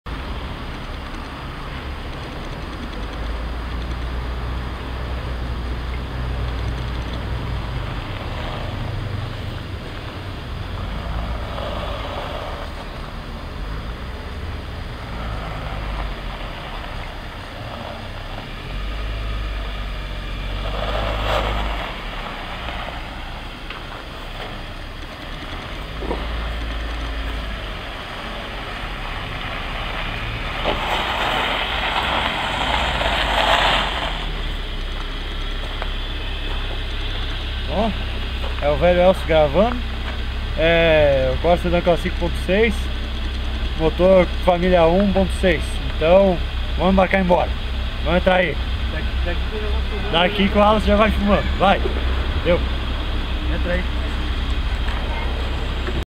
Corsa Sedan Classic 1.6 visto do lado de fora e o meu velho gravando o vídeo
Manobrando meu Corsa Sedan Classic 1.6.hehehehe e vamos comentar. desculpe o carro sujo,. mas pretendo lavar para não ter que encher o meu saco.